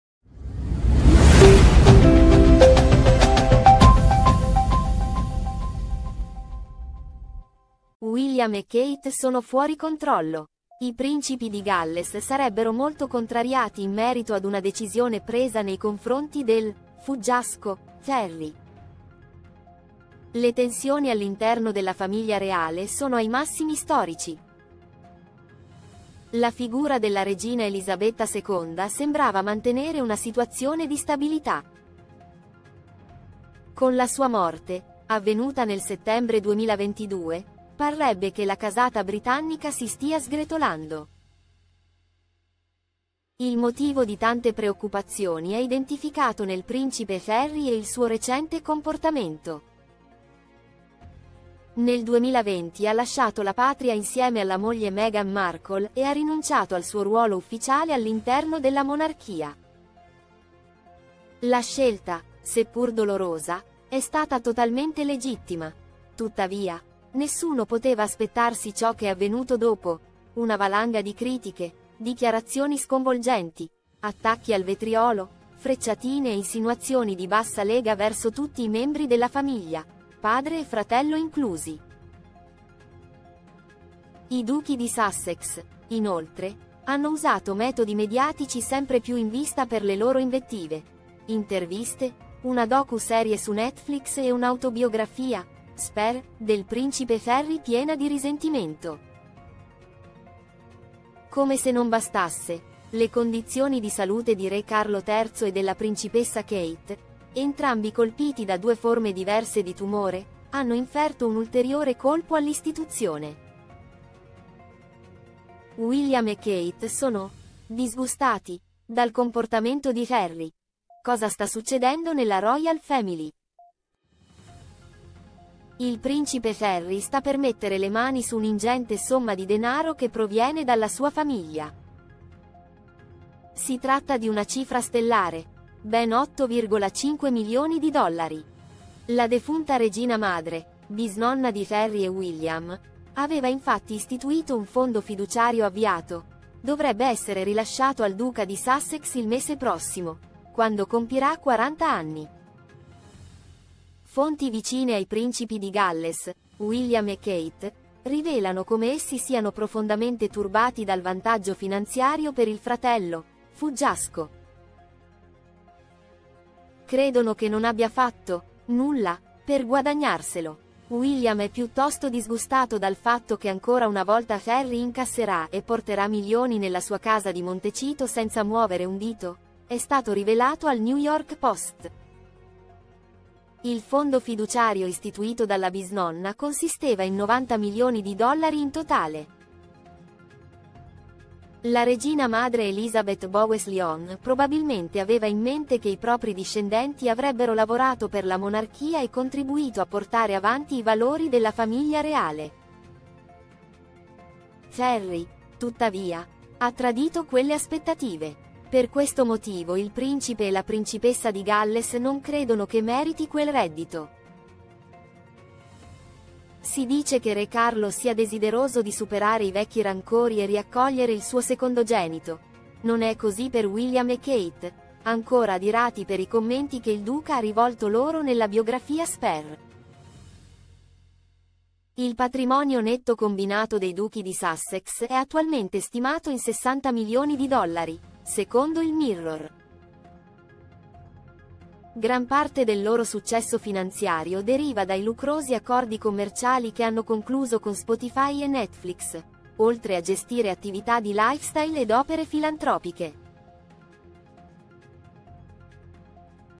Bufera nella Royal Family William e Kate furiosi, tutta colpa di Harry
Kate Middleton disgustata, William furioso: bufera a Buckingham Palace. Scoppia l’ennesima bomba nella Royal Family britannica, e questa volta pare proprio che non si possa tornare più indietro. Una situazione insostenibile anche per Re Carlo che non sa come frenare l’ennesima spaccatura insanabile.